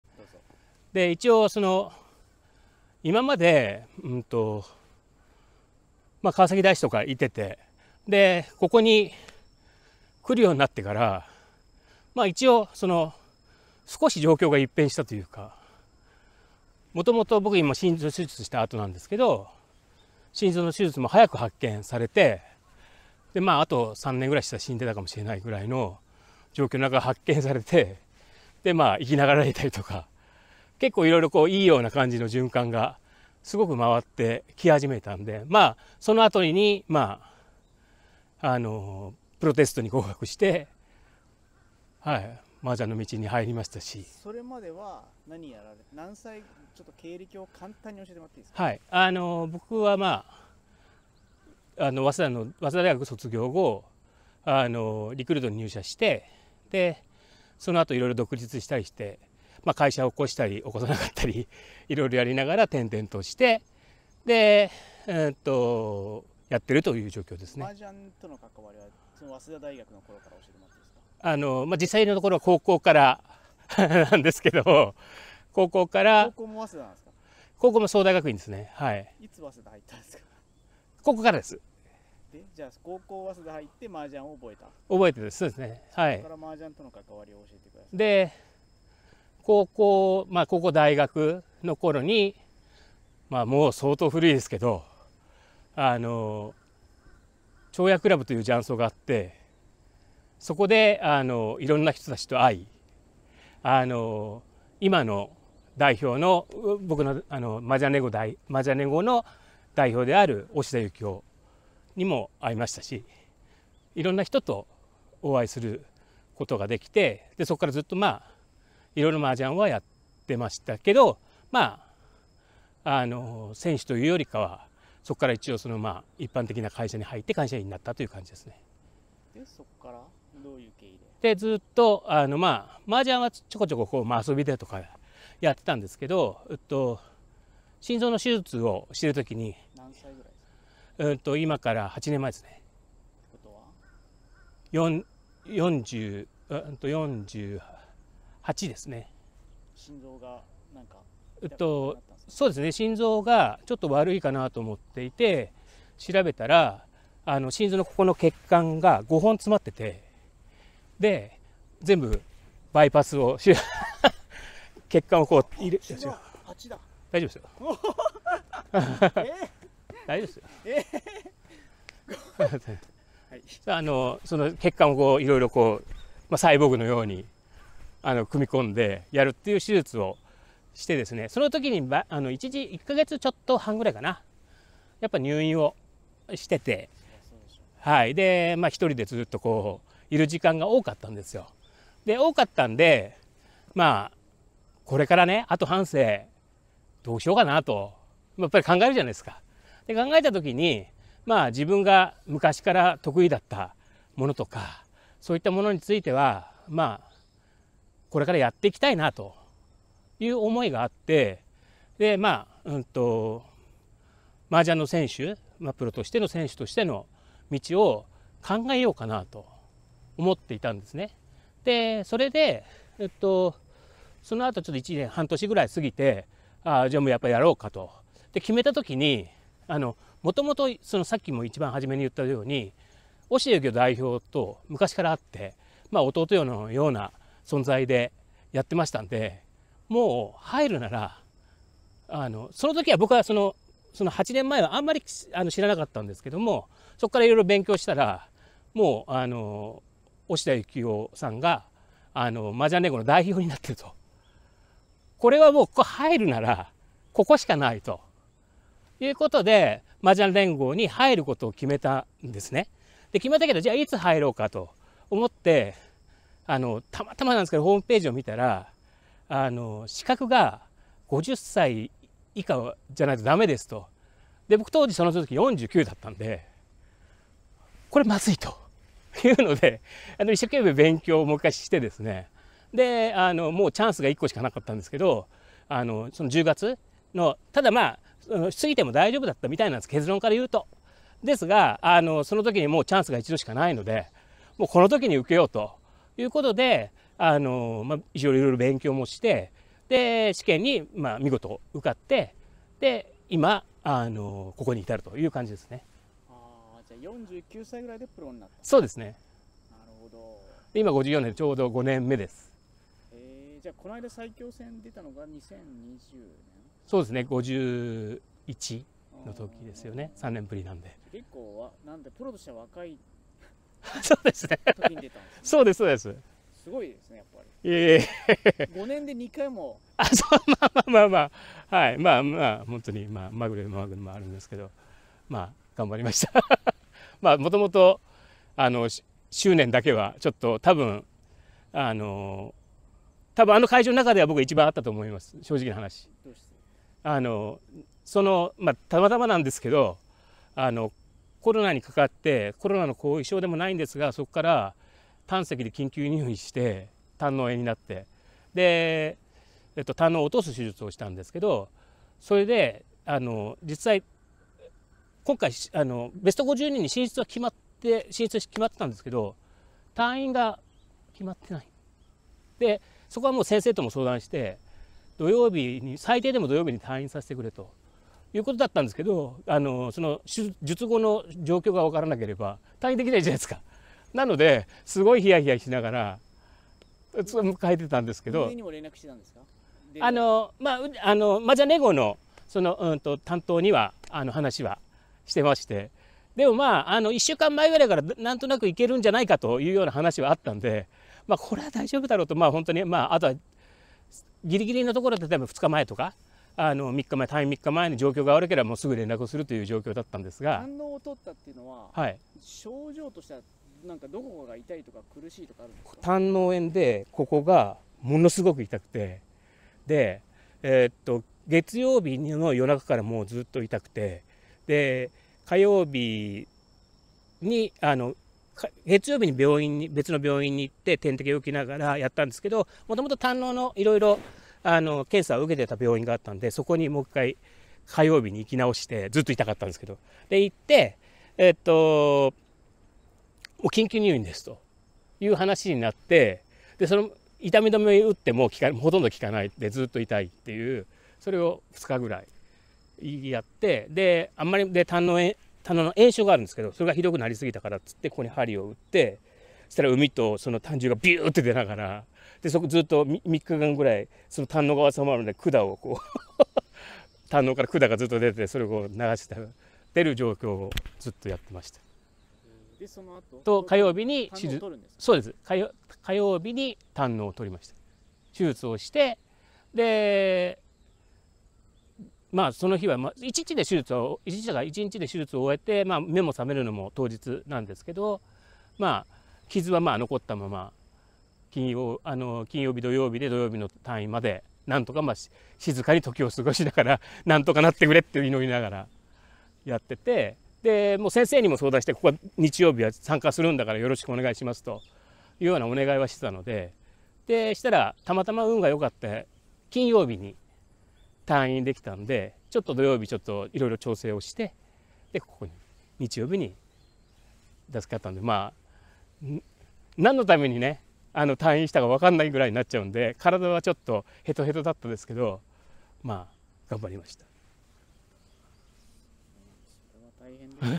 酒井俊晴プロの人生「もしかしたら生きてなかったかも」
10月15日　麻雀最強戦2023全日本プロ選手権出場

この動画は未編集です